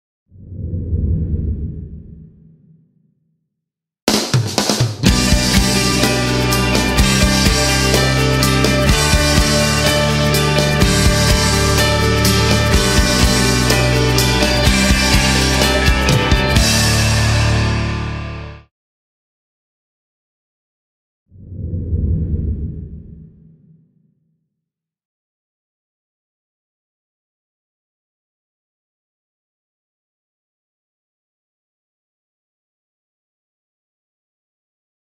Thermomix En - Experience it!
Experience Thermomix in action! Our cooking adviser will show you how all the functions work. Thermomix inspires you every day.